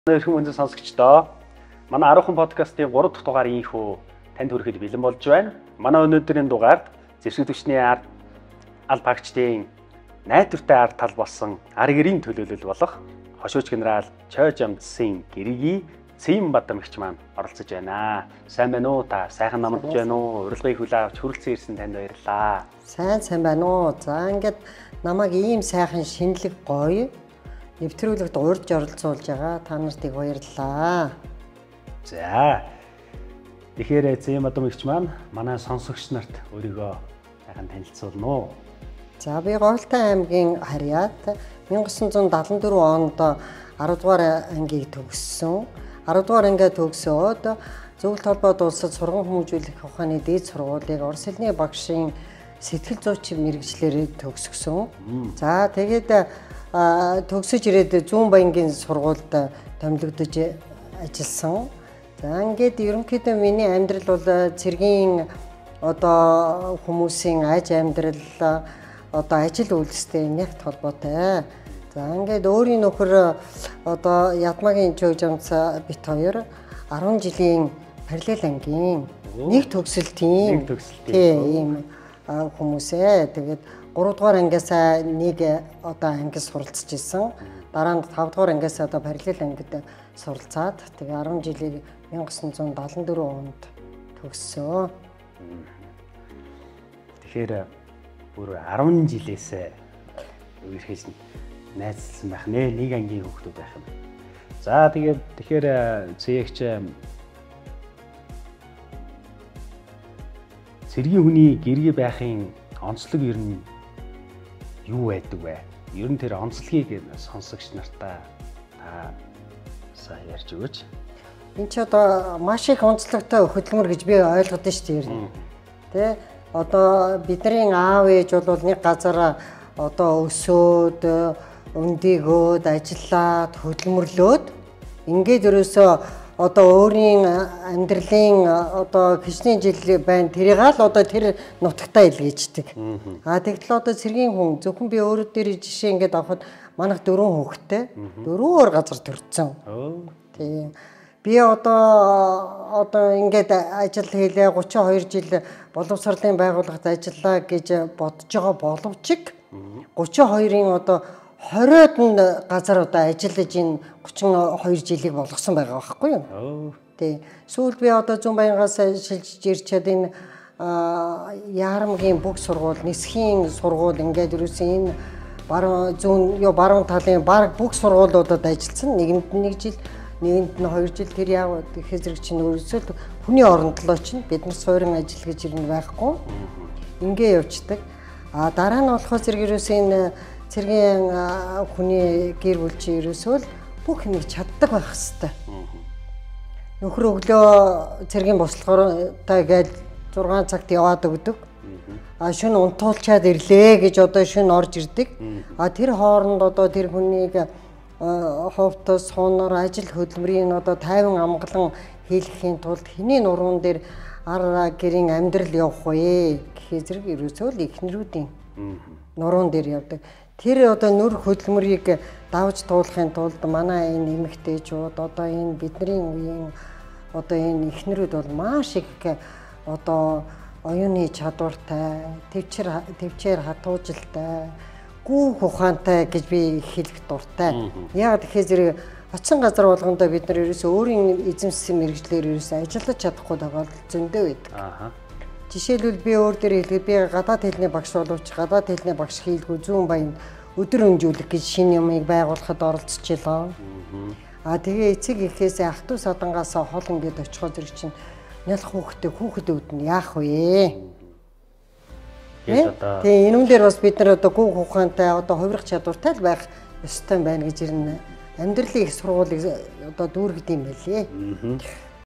EP 3.1 "Цэргийн хүний гэргий олон талын чадвар эзэмшдэг"
Хошууч Генерал Чойжамцын гэргий Ц.Цэенбадамтай хийсэн цэргийн алба хаагчийн гэр бүл болон цэргийн алба хаагчийн гэргий байхын онцлогуудын тухай ярилцлага.